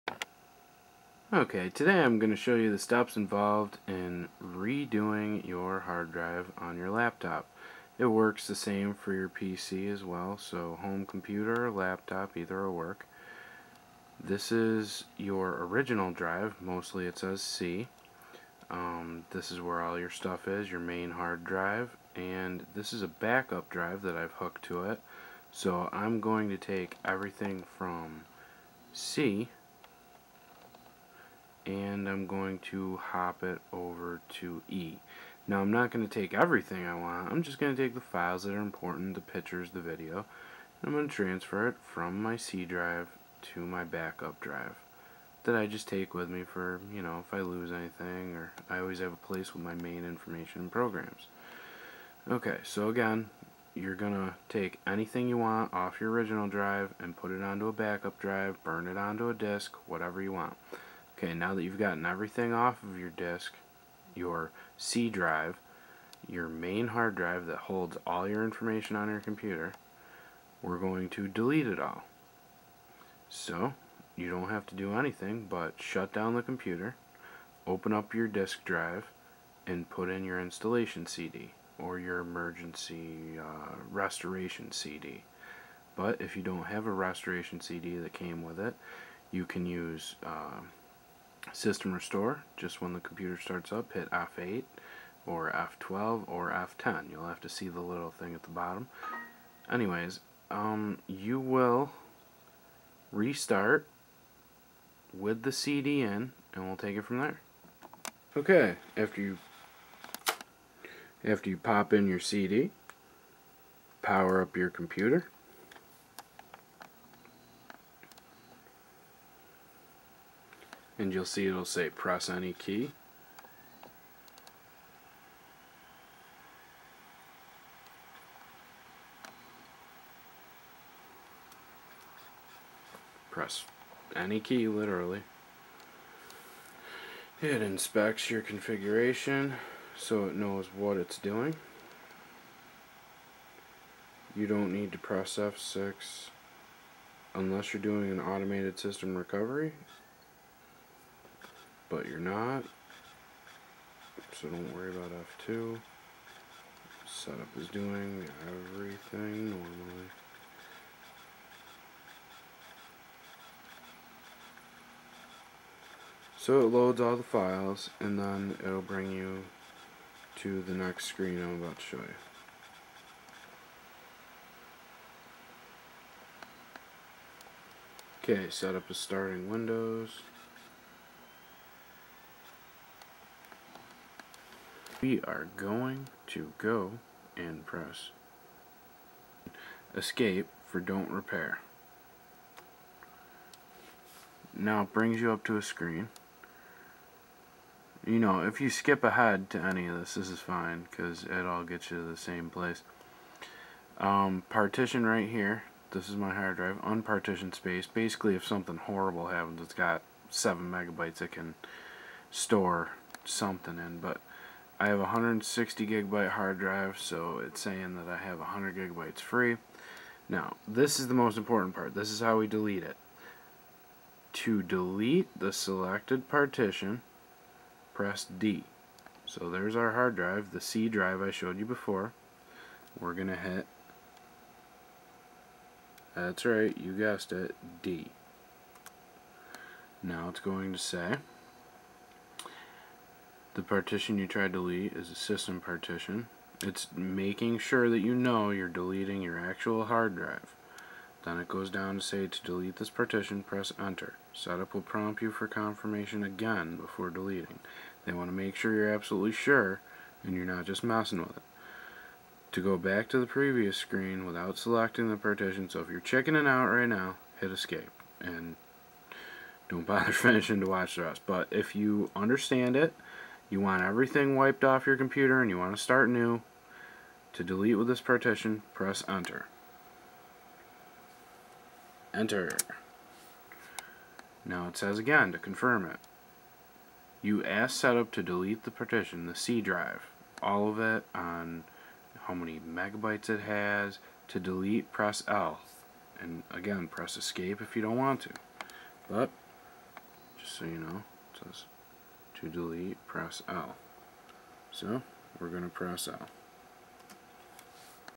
HOW TO FORMAT YOUR LAPTOP OR PC PART 1 of 2
Simple and easy detailed explanation of How to format your laptop or PC. There is ALSO A PART 2